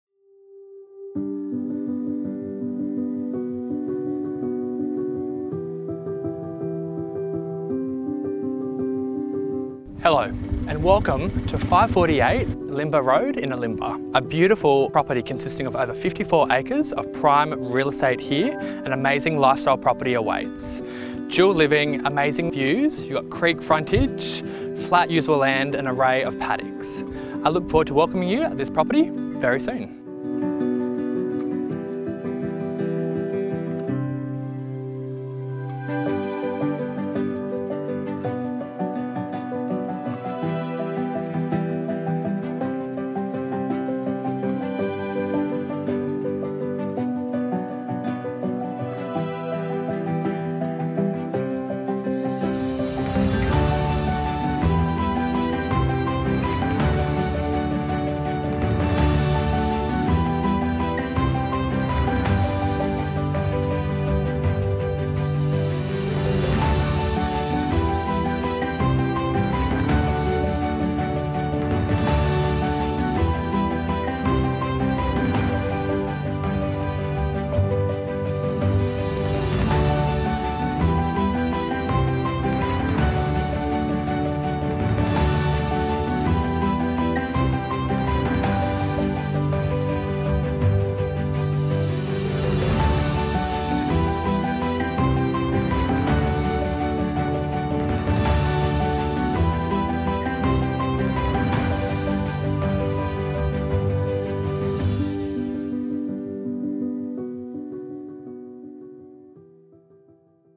548 Illinbah Road, Illinbah
A picturesque Illinbah Valley estate - offering over 54 acres of an enviable lifestyle opportunity as a full time residence or exceptional weekend getaway and within 15 minutes of the thriving township of Canungra.